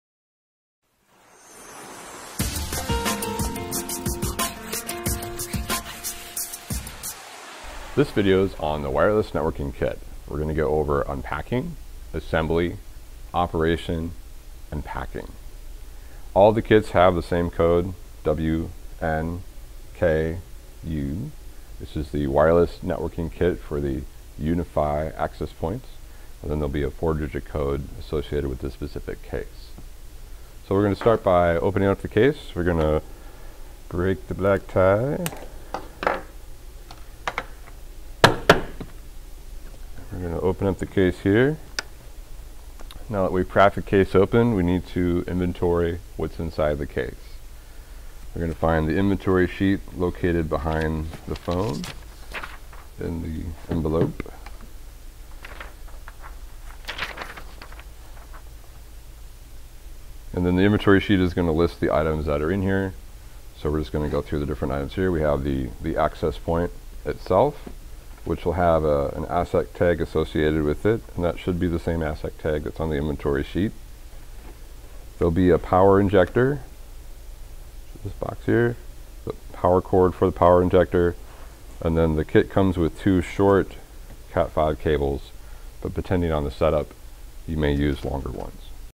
WAP Part 1 Unpacking
For optimal video quality, choose 720pHD as the YouTube player's Quality setting.

This is the first video in the playlist for the Wireless Access Point (WAP).

In this video, we review unpacking all the parts from the case and checking everything off against the inventory sheet.